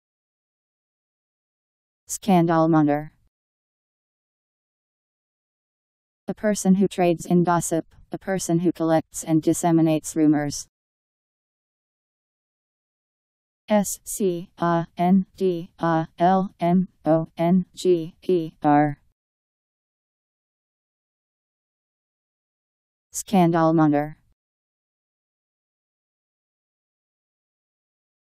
Scandalmonger Meaning
Video shows what scandalmonger means. A person who trades in gossip. A person who collects and disseminates rumors..  Scandalmonger Meaning. How to pronounce, definition audio dictionary. How to say scandalmonger. Powered by MaryTTS, Wiktionary